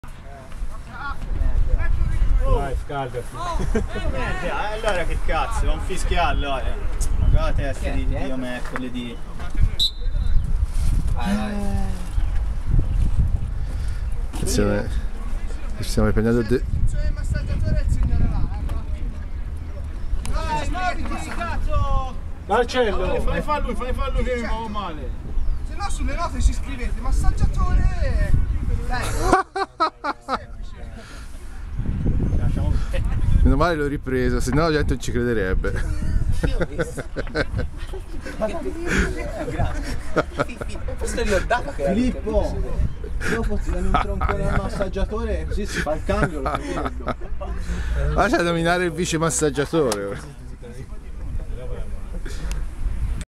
San Marco - FC Lucca. L'arbitro ha sbagliato categoria
San Marco - FC Lucca, campionato amatori aics IV serie. L'arbitro crede di arbitrare la Tim Cup... e vuole che a prestare soccorso ad un infortunato sia solo quello che sulla distinta dei giocatori è segnato come massaggiatore. Forse dovrebbero spiegargli in AICS Lucca che nessuna squadra amatori c'ha il massaggiatore... (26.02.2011)